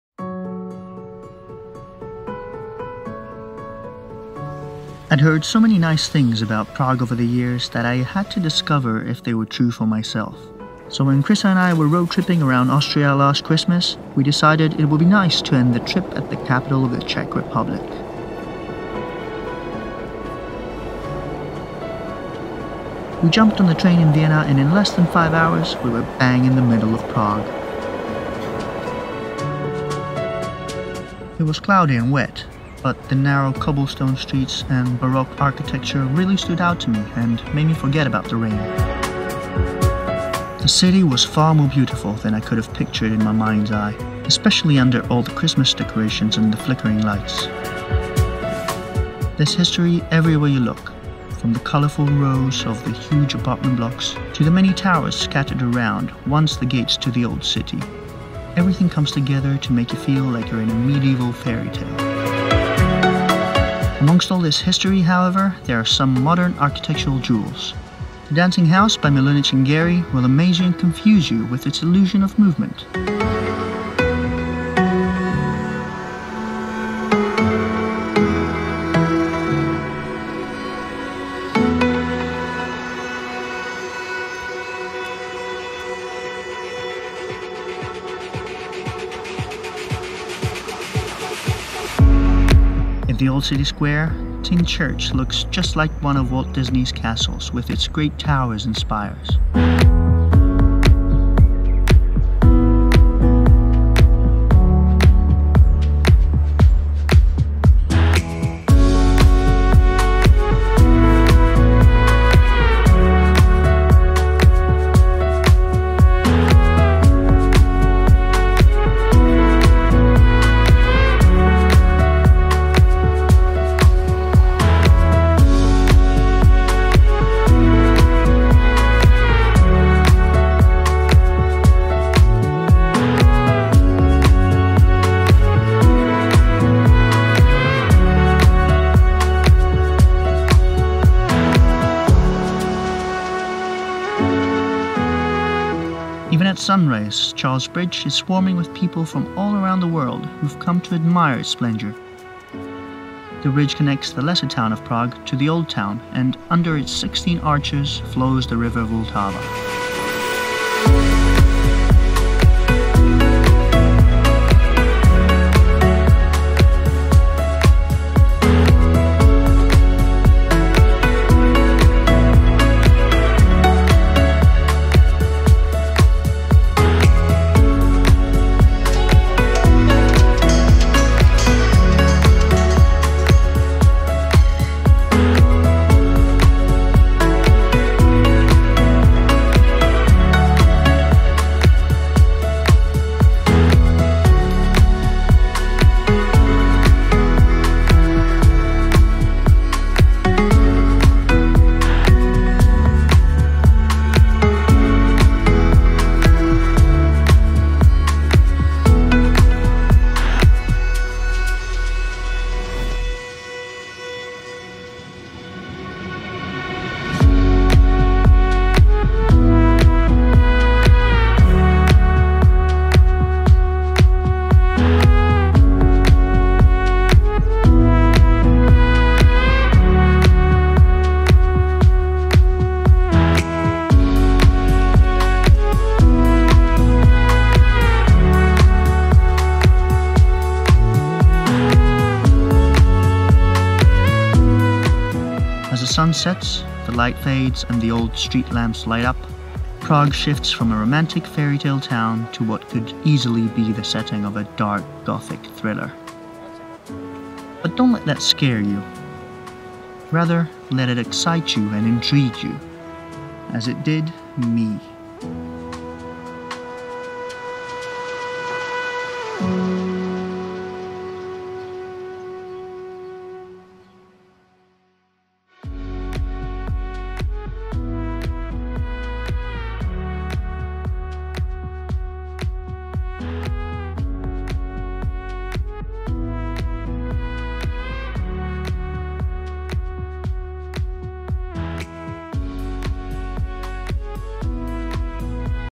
Prague, the Fairytale City // Explore Europe
02nd to 5th of January 2018 // Prague, Czech Republic

To end our roadtrip around Austria we jump on a train from Vienna to Prague and spend three magical days in this fairytale-like European City.